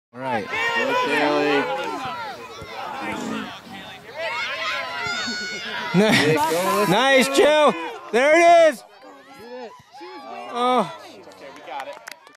Jillian Plays a Nice Ball and Alyssa Makes a Nice Run
11/12/11 Samba vs FC Diablo Black
at Creekside Park

Samba win 2-1